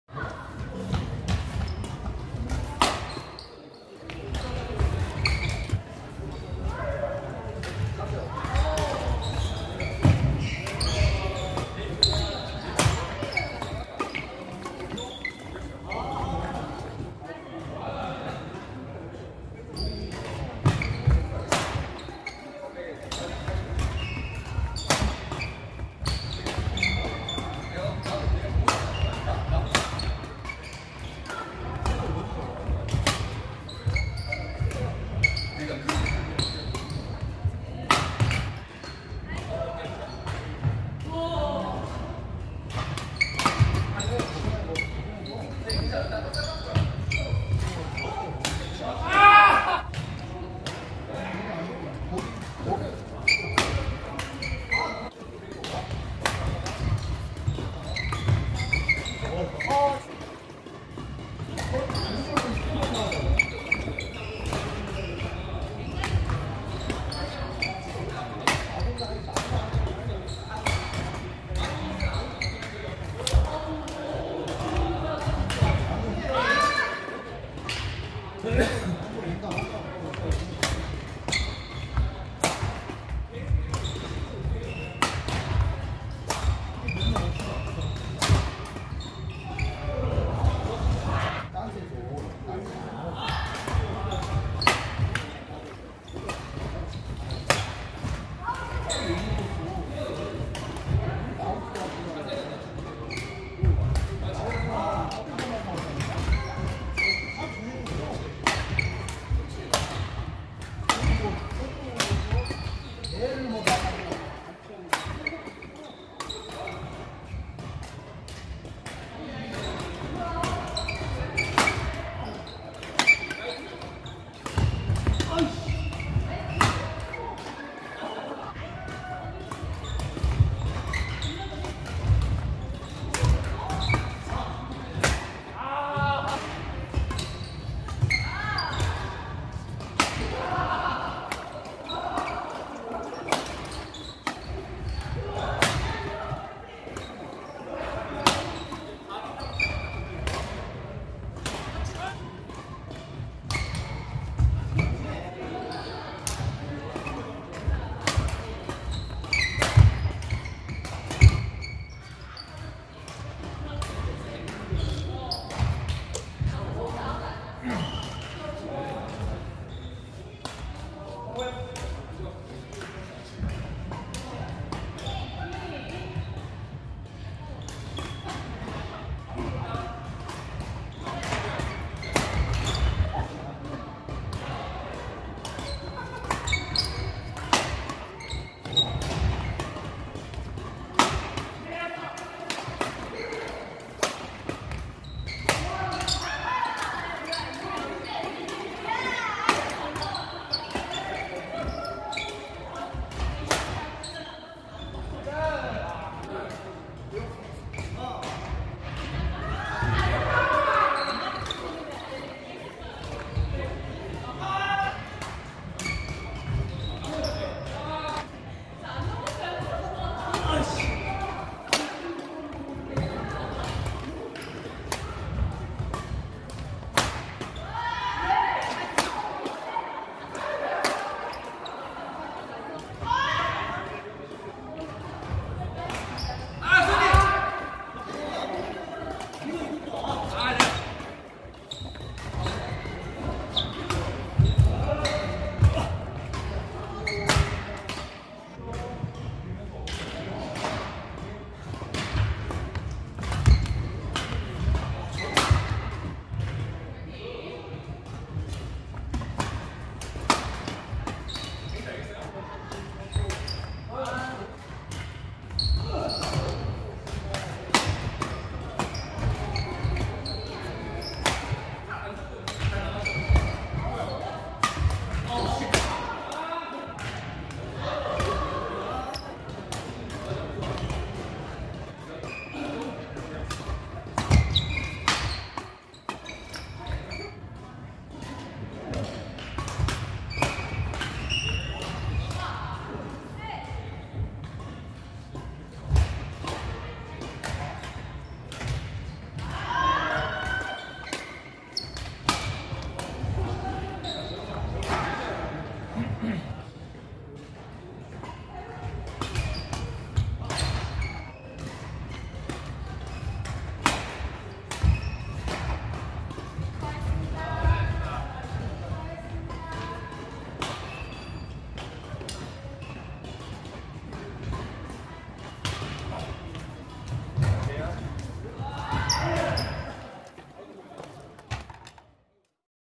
배드민턴TV(with.김별 최재훈 정상철 동호인)
서울시 관악구에 위치한 국사봉 배드민턴 전용 체육관입니다.

코치
1.김별코치님은 실업팀인 화순군청출신으로 현재     문창클럽에서 레슨 진행 중 이시구요.
다수의 코치 및 a~c조까지 모임 팀 코드네임 소속이십니다~

매주 화,목 빡시게 경기를 하며 배우고있습니다!

구독 및 좋아요~